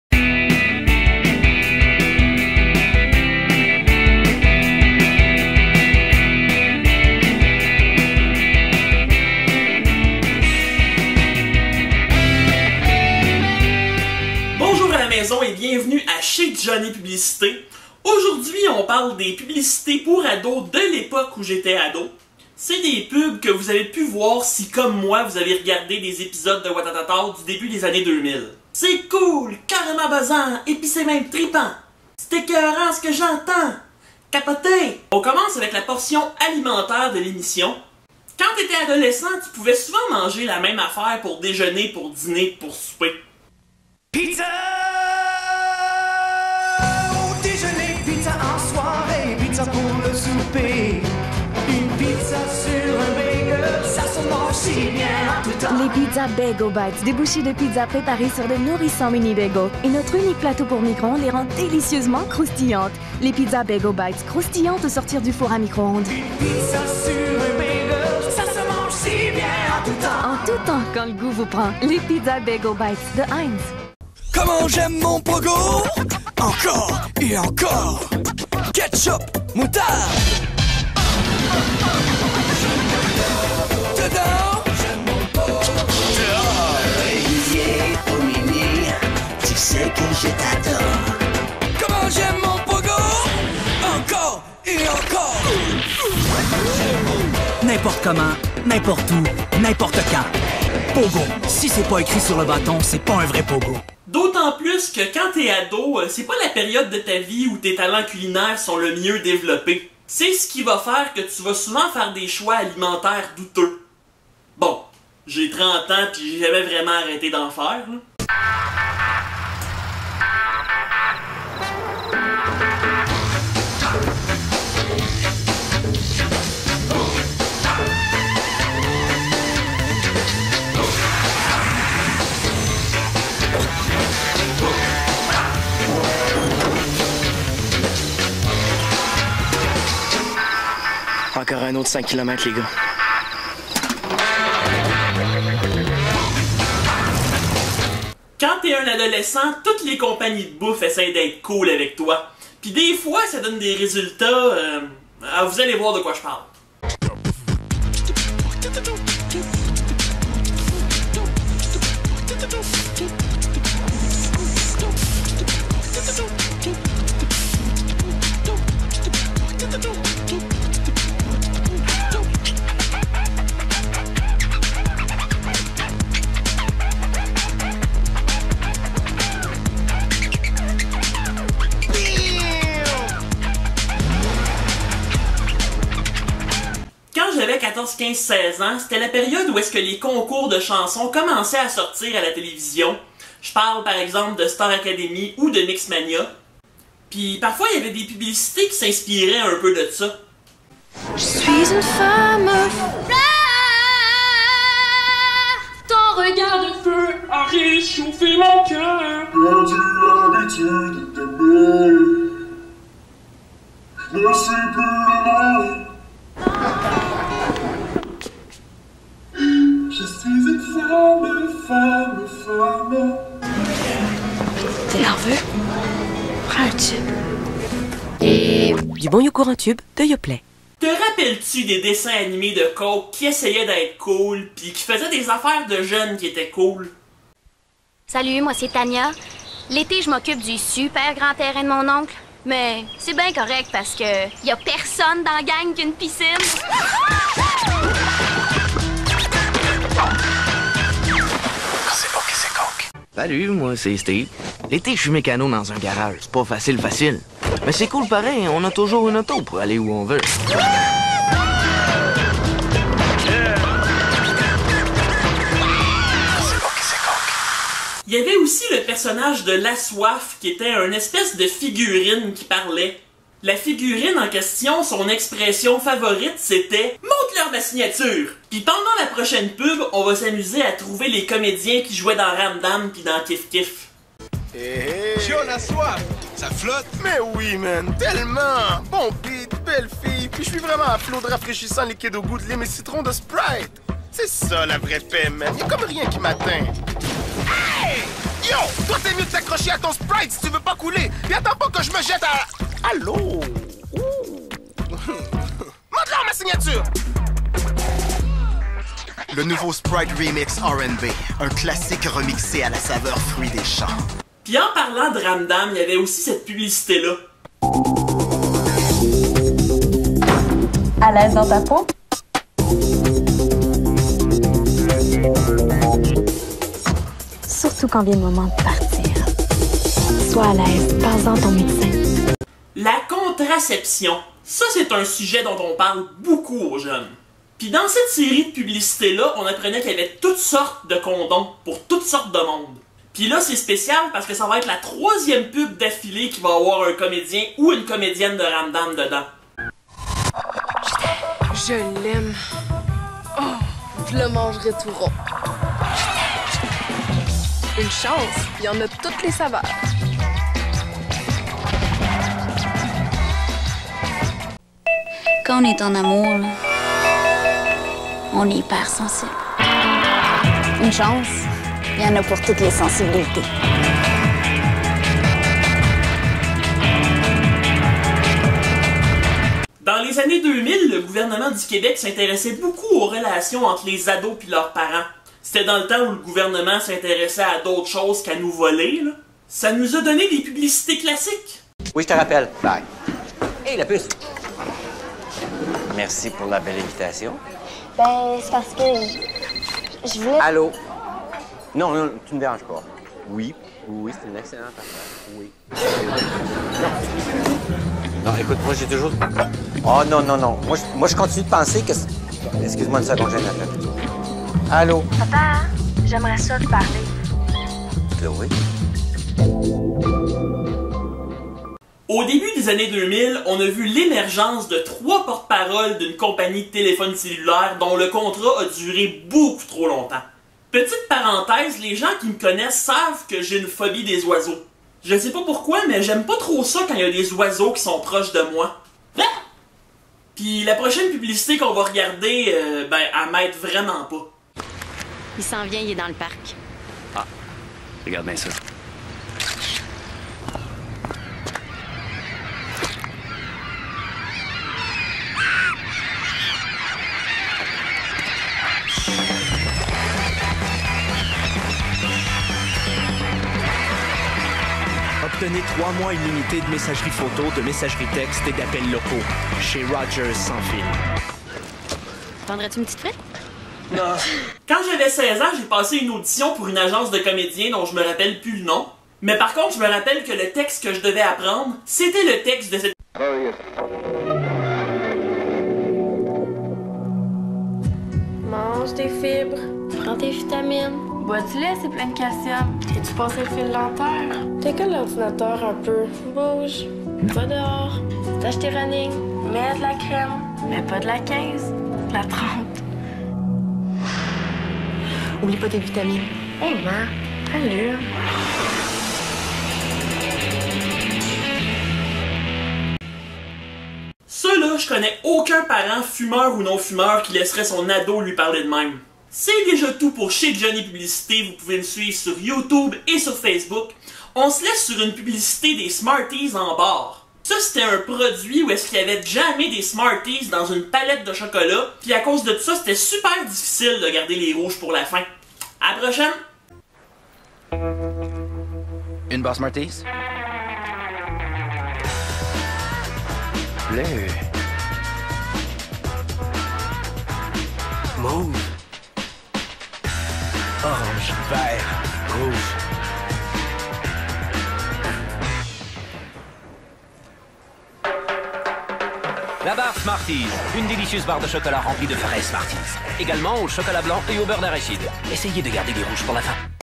Chez Johnny Publicité S03E08 - Pubs pour ados de quand j'étais ado
Le titre dit tout, on va aller regarder des pubs de la bonne vieille époque où je regardais Watatatow en revenant de l'école secondaire. Au menu, des pubs de condoms, de Pogo, de Bagel Bites, des pubs de liqueurs avec le personnage de La Soif, plein de comédiens de Ramdam et de Kif-Kif, des pubs anti-tabac avec la gang Allumée, les dessins animés de Coke, les trois ados de 30 ans de la pub de Rogers et plein de belles surprises.